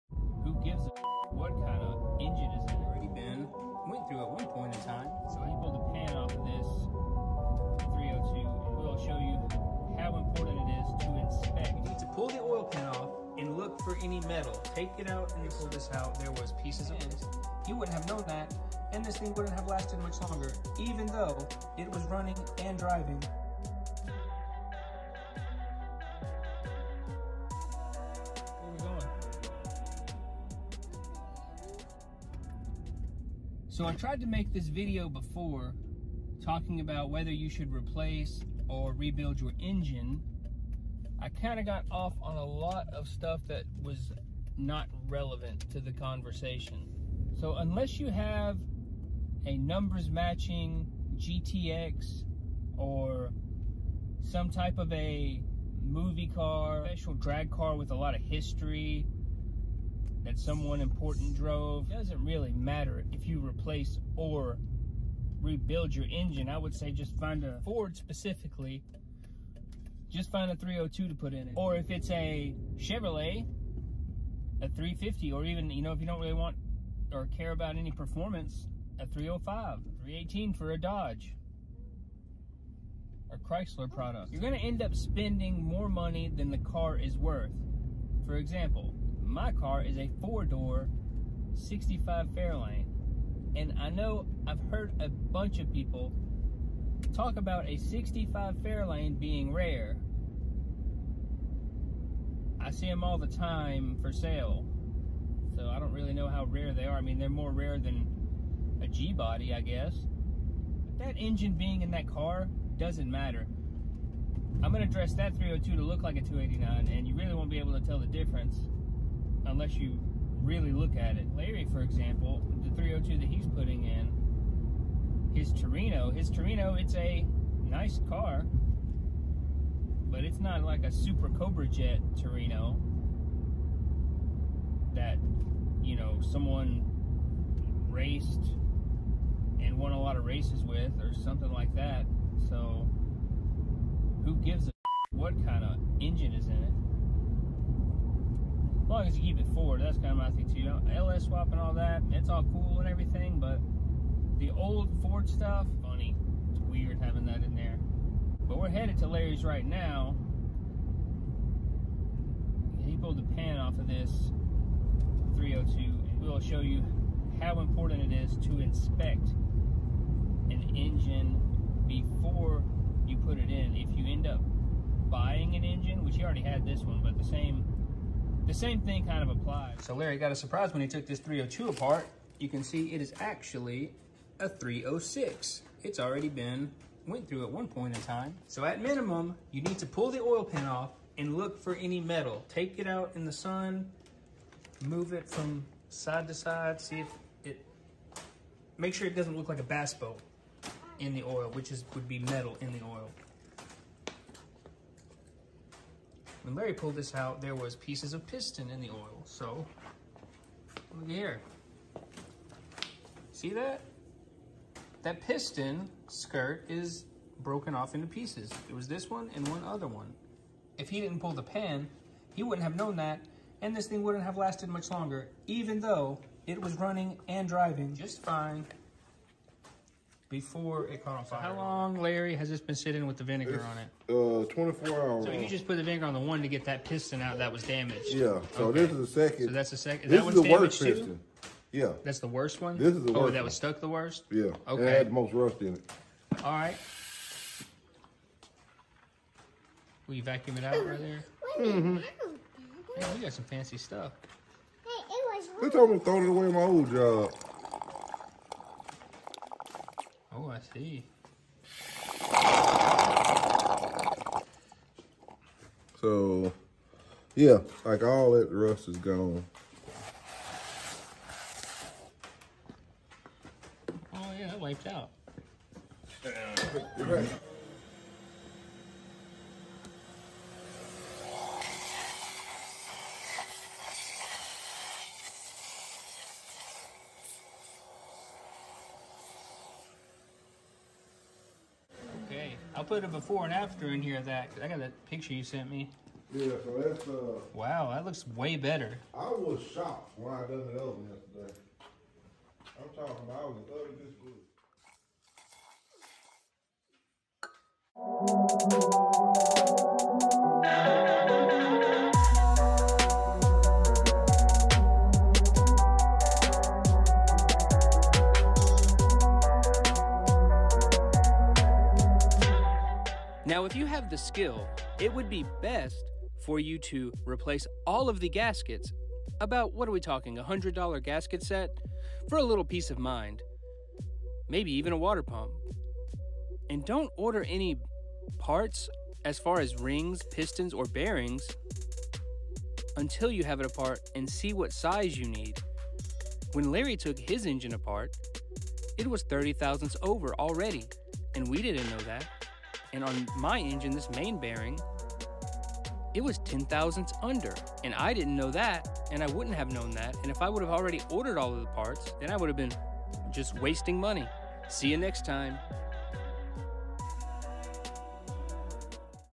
Should you rebuild or replace your engine? Pt. 2. The importance of inspecting a used engine.
Dont beleive the seller “Ran when pulled” lol. Unless you have something very rare and valuable, or just want to rebuild the engine. Replacement is likely your best option, but you need to at least look at it and give it a once over inspection before installation, or you may be sorry.